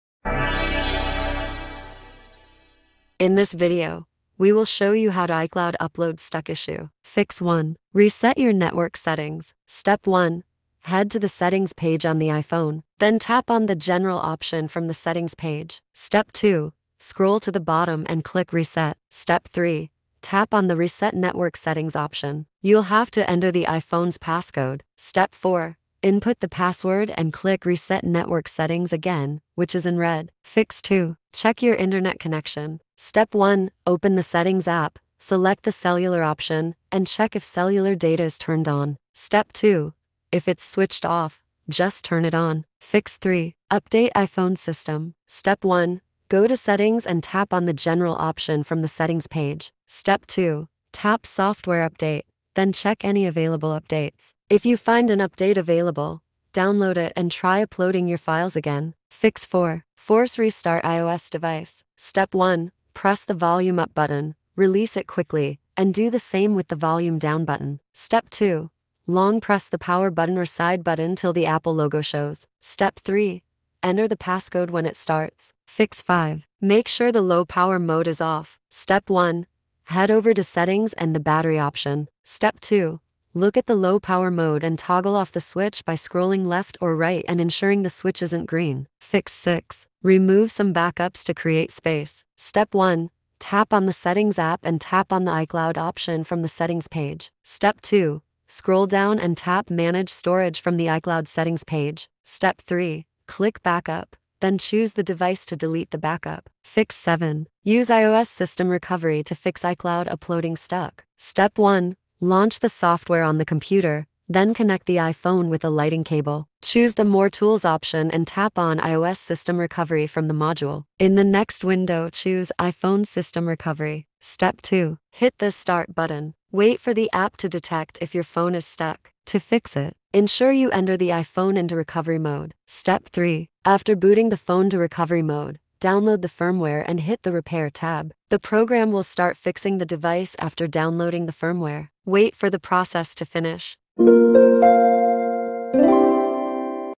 How to Fix iCloud Upload Stuck Issue [7 Common Solutions]
To solve iCloud upload stuck, you can try some basic solutions. If these solutions don't work out for you, use iOS System Recovery, which fixes this issue efficiently.

00:00 Start
00:03 Fix 1. Reset Your Network Settings
00:30 Fix 2. Check Your Internet Connection
00:42 Fix 3. Update iPhone System
00:58 Fix 4. Force Restart iOS Device
01:15 Fix 5. Make Sure the Low Power Mode is Off
01:28 Fix 6. Remove Some Backups to Create Space
01:46 Fix 7. Use iOS System Recovery to Fix iCloud Uploading Stuck